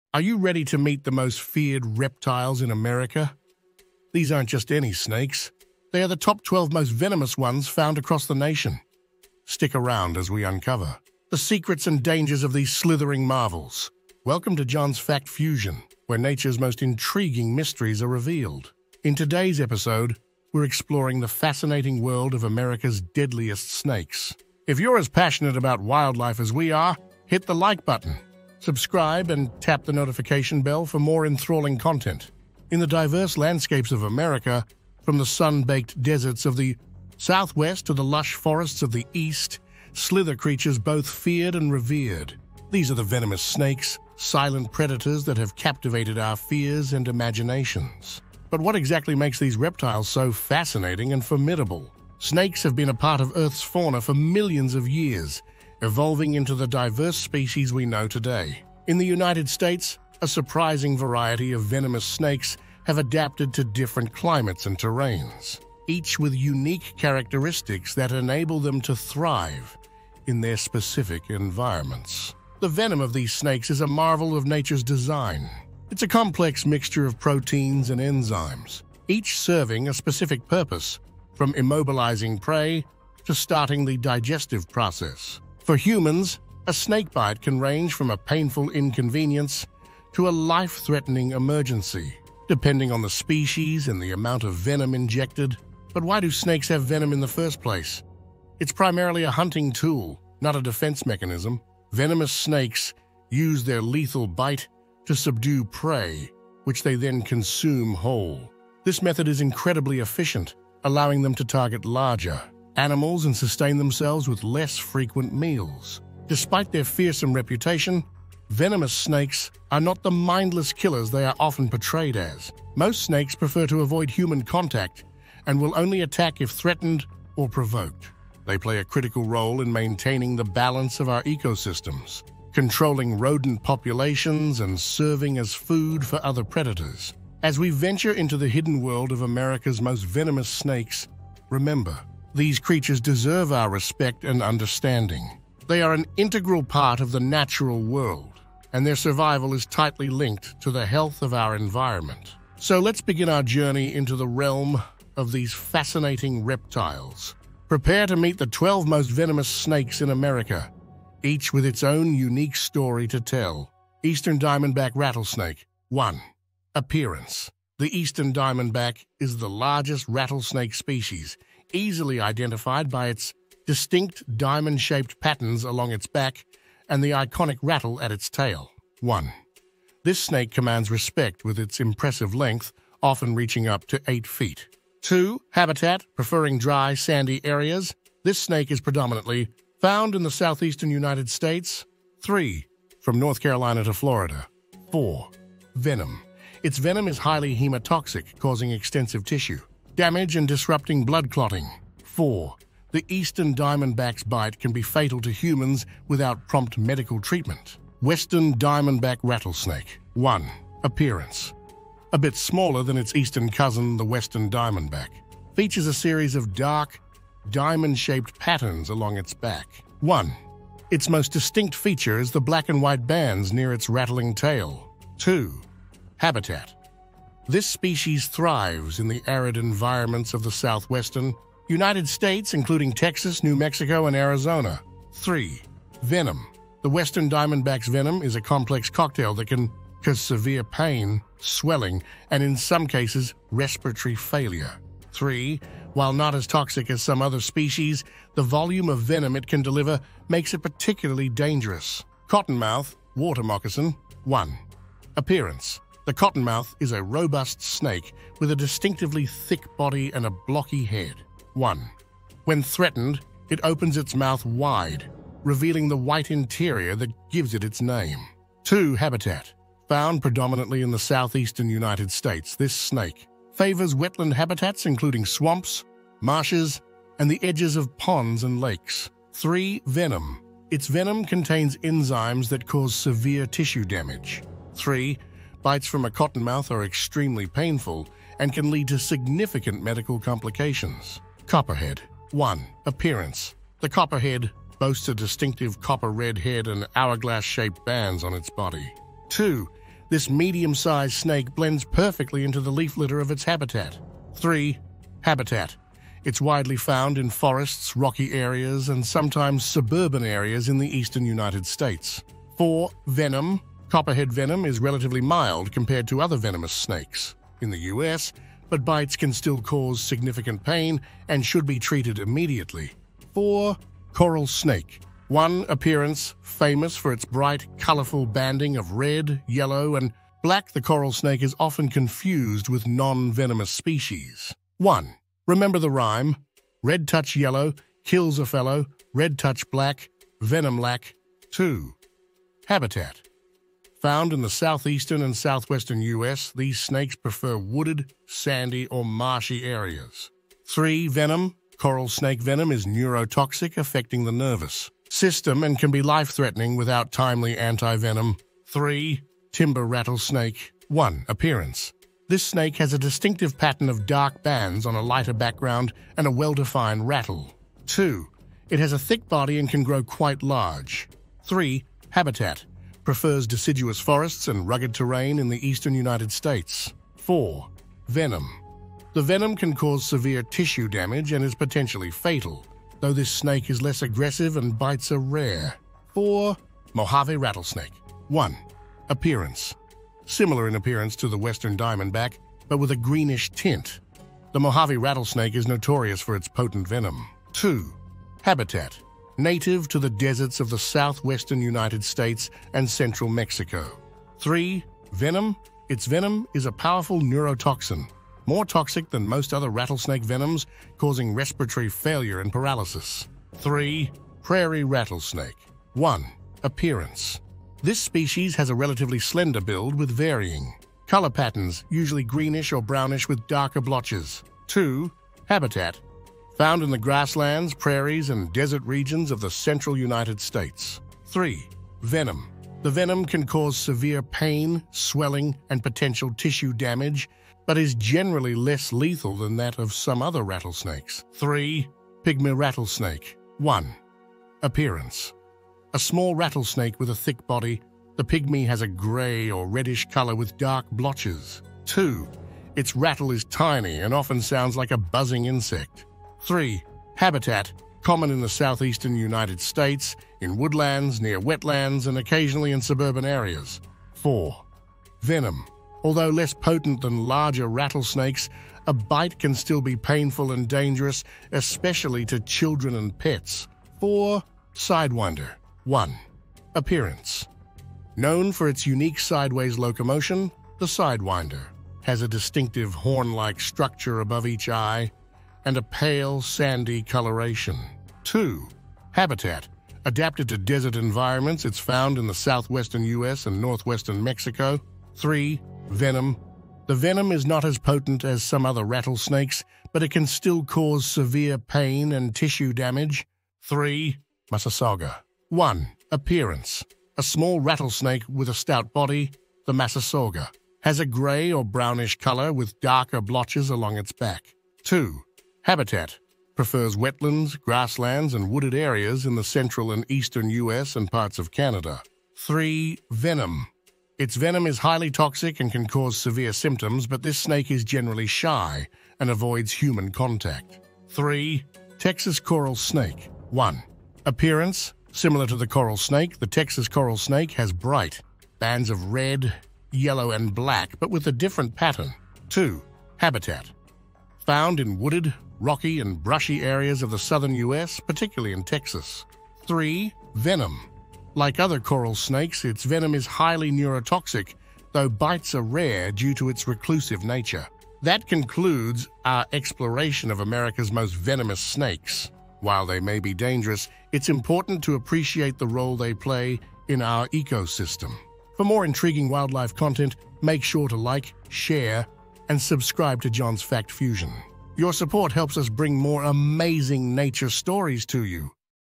"🚨 DANGER LURKING IN YOUR BACKYARD? 🚨 America’s 12 DEADLIEST Snakes Revealed - #9 Will Shock You!"
"Think you're safe in your own backyard? Think again! We've uncovered America's 12 MOST VENOMOUS snakes, and the facts are startling! 🐍 From the lethal bite of the Eastern Diamondback to the surprising danger of the Pygmy Rattlesnake, this video will reveal the hidden threats slithering in the shadows. Watch NOW to discover which deadly serpent might be lurking near YOU! 🚨 SnakeDanger WildlifeRevealed StaySafe"